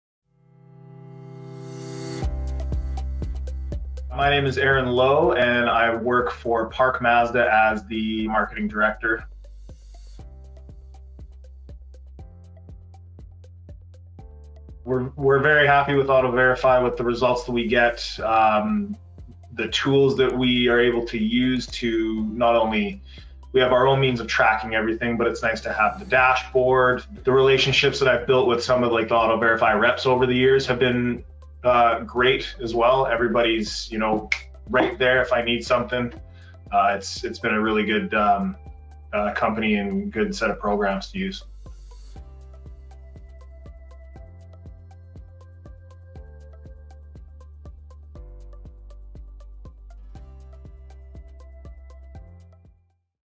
Tell us why you love AutoVerify | AutoVerify Customer Story: Park Mazda
Hear what Aaron Lowe at Park Mazda likes most about AutoVerify. Find out what AutoVerify can do for your dealership today